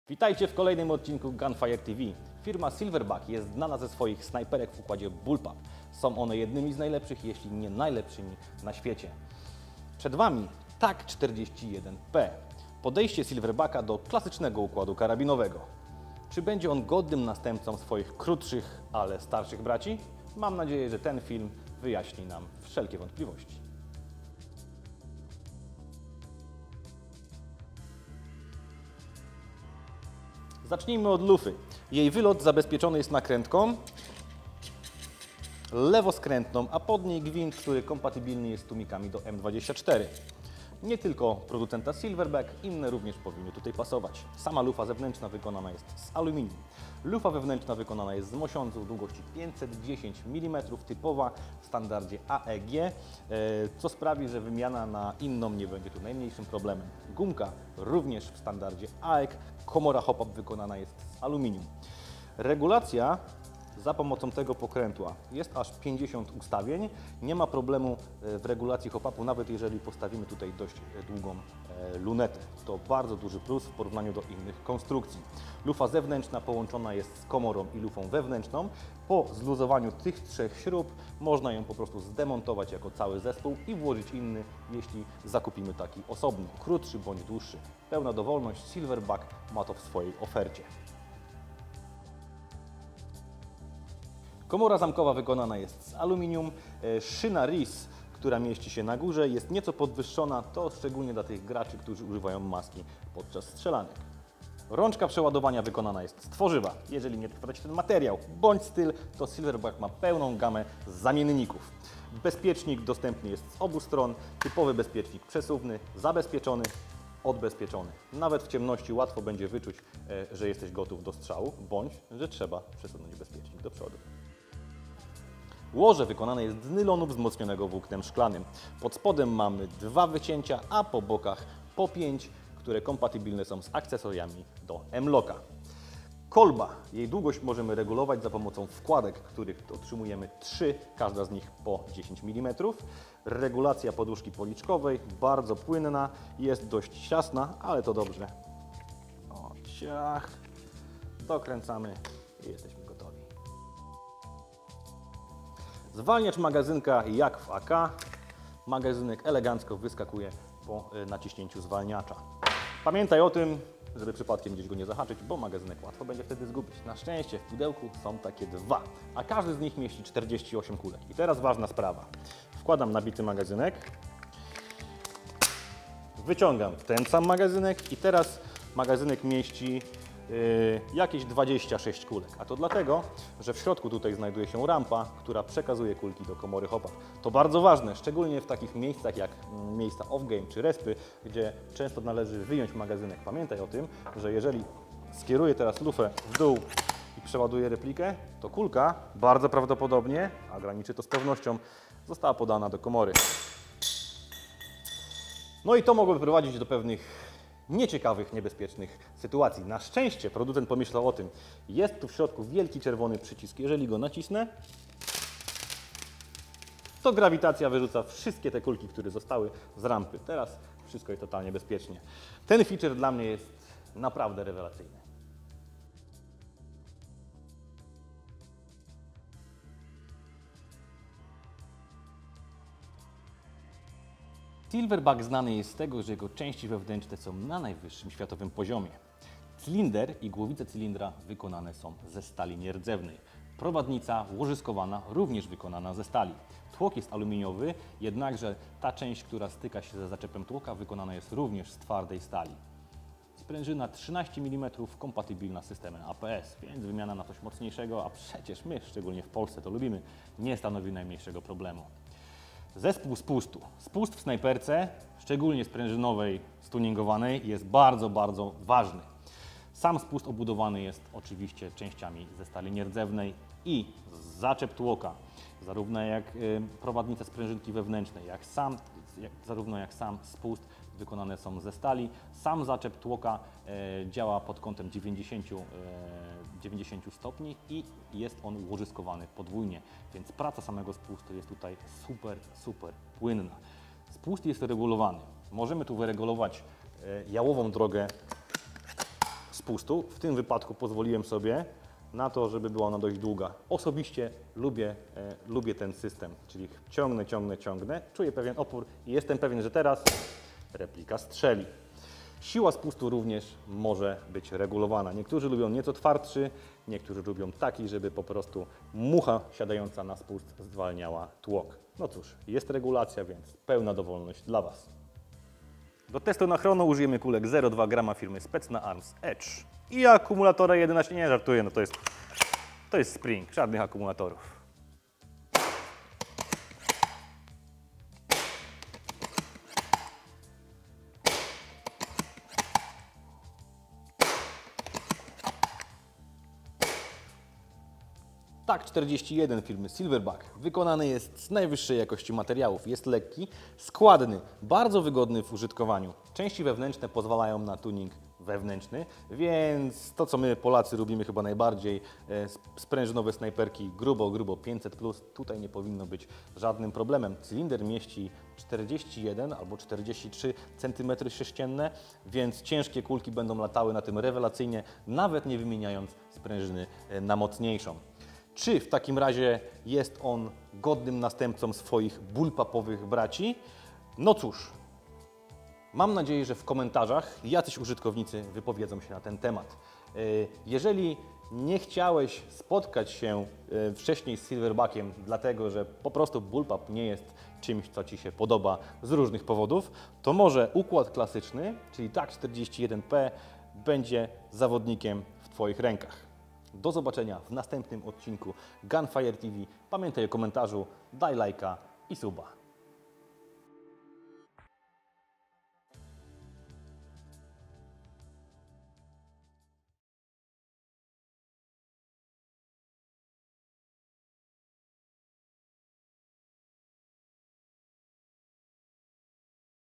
Tac-41 od Silverback - Król snajperek ASG?
Repliki karabinów snajperskich niezmiennie cieszą się dużą popularnością. Silverback Airsoft osiągnął mistrzostwo w kwestii ich projektowania. Leszek nie mógł Was zostawić bez recenzji repliki. Na uwagę zasługuje wiele rozwiązań zastosowanych w replice ASG. 50-cio stopniowa regulacja Hop-Up, bardzo dokładny, regulowany spust czy wygodny dostęp do cylindra czynią z Tac-41 ciekawą snajperkę ASG. Pozycja obowiązkowa dla fanów replik z najwyższej półki. Na premierę będziemy musieli poczekać, ale już wiemy, że warto!

0:00 - Wstęp
0:24 - Prezentacja
1:01 - Regulacja Hop-Up
1:15 - Demontaż lufy zewnętrznej
1:35 - Komora i szyna
2:05 - Łoże
2:15 - Kolba
2:35 - Magazynek
2:50 - Podajnik kulek w replice
3:55 - Części wewnętrzne
4:40 - Spust
5:24 - Test Chrono
5:48 - Wrażenia Leszka
6:40 - Zakończenie